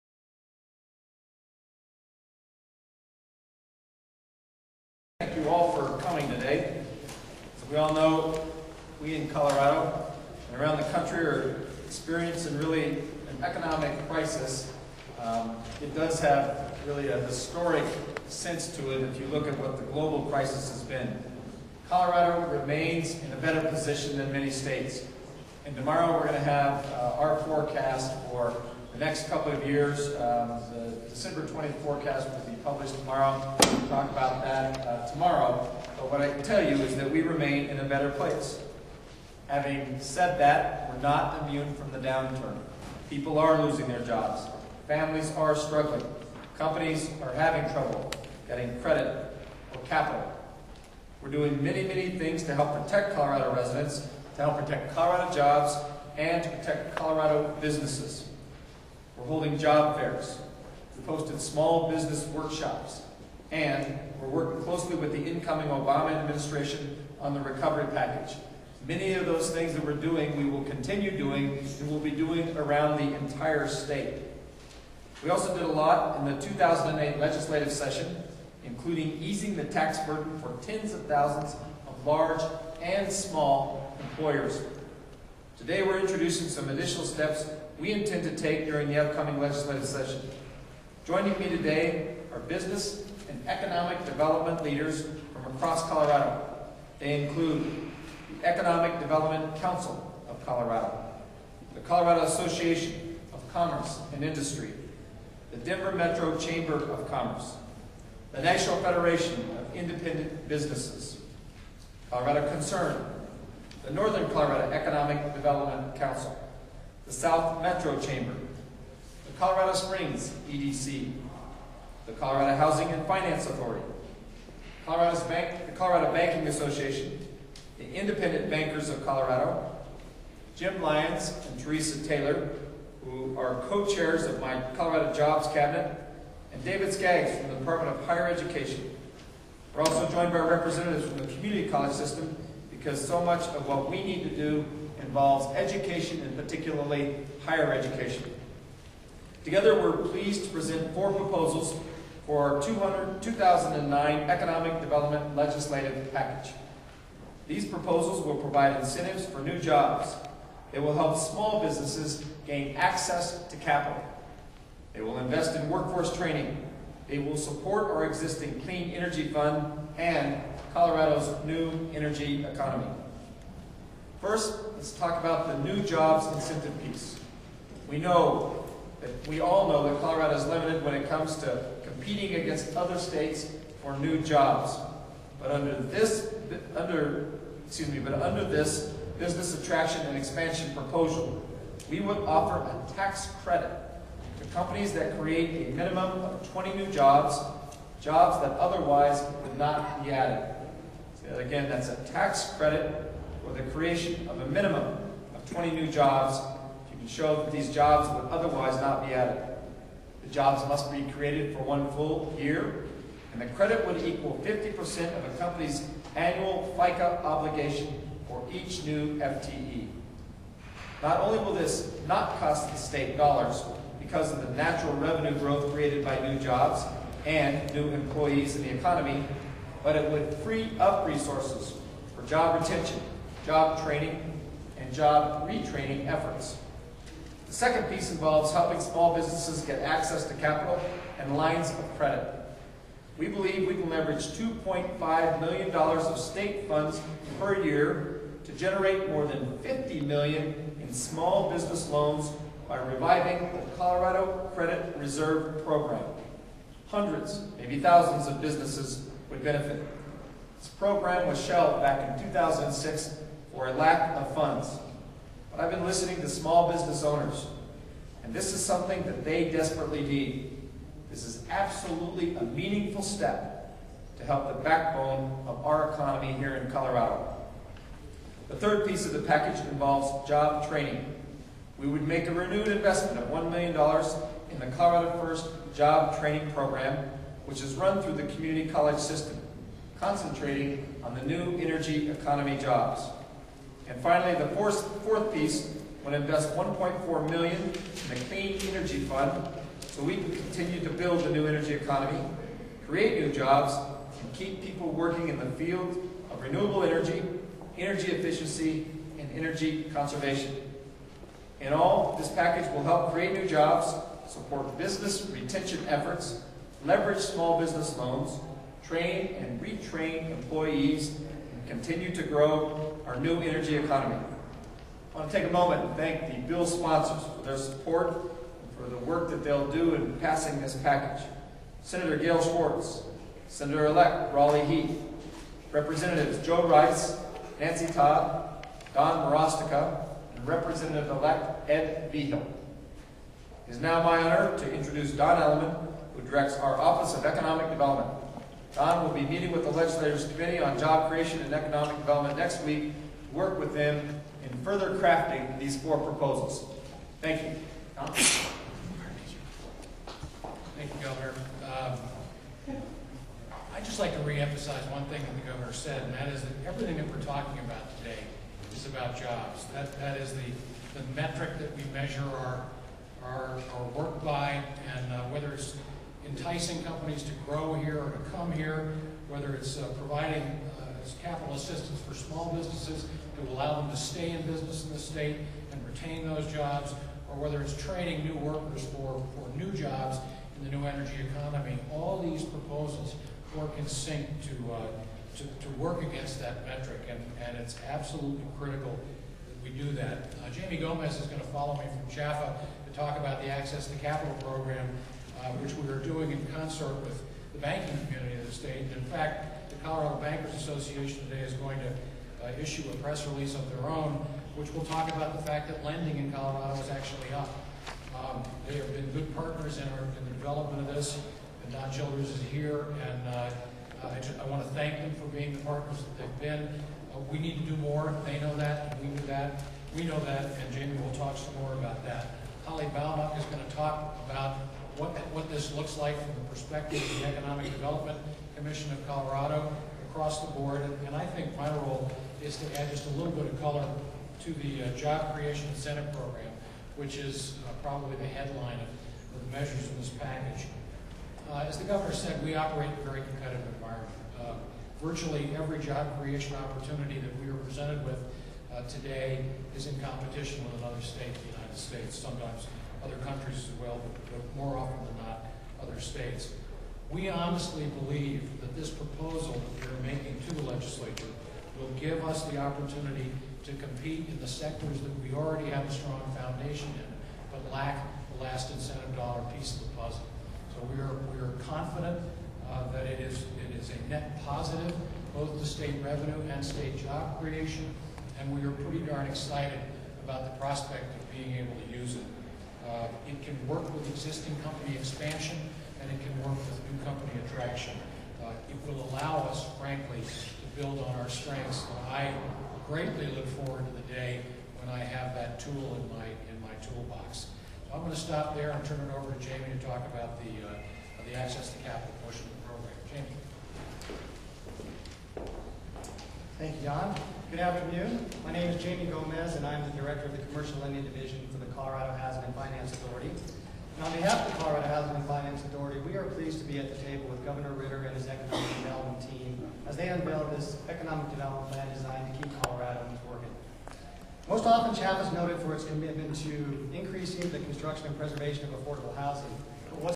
Colorado Gov. Bill Ritter Economic Announcement Dec. 19, 2008 Part 1
OFFICE OF GOVERNOR BILL RITTER, JR

THURSDAY, DEC. 18, 2008

GOV. RITTER ANNOUNCES 2009 ECONOMIC PROPOSALS

Joined by statewide business leaders and a bipartisan group of lawmakers, Gov. Bill Ritter today announced his job-creation and economic-development proposals for the upcoming 2009 legislative session, which will:

Provide tax-credit incentives for companies that create at least 20 new jobs.
Provide hundreds, possibly thousands, of small and family-owned businesses with immediate access to capital and credit through a Colorado Credit Reserve Program.
Expand Colorado First workforce training programs at community colleges, particularly for New Energy Economy jobs in renewable energy, energy efficiency and energy conservation.
Invest in New Energy Economy companies, projects and jobs through the Colorado Clean Energy Fund.

"As we all know, we're experiencing a historic and a global economic crisis," Gov. Ritter said. "We're doing better than many states, but we're not immune and we face steep challenges. People are losing their jobs. Families are struggling. Companies are having trouble getting needed capital or credit to keep their doors open.

"Over the past two years, we¿ve made smart, strategic and aggressive investments to help protect Colorado residents and retain Colorado jobs and businesses," Gov. Ritter added. "Our 2008 business-development legislative package eased the tax burden for tens of thousands of companies, we created a Colorado Jobs Cabinet, and the proposals we are announcing today will build on the progress we have made and create new opportunities.

"These proposals will make Colorado more competitive for new jobs. They will help small businesses get through this crisis, and they will allow the state to provide a modern workforce for the 21st century New Energy Economy."

Business and economic-development organizations supporting the proposals include:

Economic Development Council of Colorado

Colorado Association of Commerce and Industry

Colorado Concern

Colorado Chapter of the National Federation of Independent Businesses

Northern Colorado Economic Development Council

South Metro Denver Chamber of Commerce

Adams County Economic Development

Colorado Springs Economic Development Council

Colorado Housing and Financing Authority

Colorado Bankers Association

Independent Bankers of Colorado

Legislators who joined Gov. Ritter at today's news conference or will be sponsoring pieces of the business-development package include Sen. Gail Schwartz, Sen.-elect Rollie Heath, Rep. Joe Rice, Rep. Nancy Todd, Rep. Don Marostica and Rep.-elect Ed Vigil.

Don Elliman, director of the Governor's Office of Economic Development, will present the business-development package to the legislature's Committee on Job Creation and Economic Growth on Monday.

Summaries of the four proposals:

Job Growth Incentive Program: Gov. Ritter is proposing the creation of a new tax-credit program to incentivize businesses to create jobs in Colorado. For a company to qualify for this program, the company must apply to the Economic Development Commission and meet specific criteria. The company will be eligible for a tax credit of up to 50 percent of its annual FICA taxes on new employees. The tax credit for new employees would be calculated on a year-to-year basis for five years according to the number of FTEs on the company's payroll at year's end.

Colorado Credit Reserve Program: Colorado Credit Reserve is a pool guaranty program originally established and administered by the Colorado Housing and Finance Authority. Gov. Ritter is proposing that this program be restored and expanded to generate small business loans throughout the state. The revitalized program is designed to encourage lenders to extend credit amounts up to $500,000 to Colorado businesses. By committing $2.5 million to this program at a maximum of 2.5 percent per loan (the percentage varies according to loan size), the state could leverage more than $50 million in new loans.

New Energy Jobs Program: Gov. Ritter is proposing to increase funding by $1 million per year to this program, which will be targeted for new energy job training programs.  These new funds will be used to create job training programs for new energy jobs and will allow community colleges to apply for cluster-based grants.

Clean Energy Fund: Gov. Ritter is proposing to increase current funding to the Clean Energy Fund by $1.4 million, with priority given to economic development incentives for clean energy companies.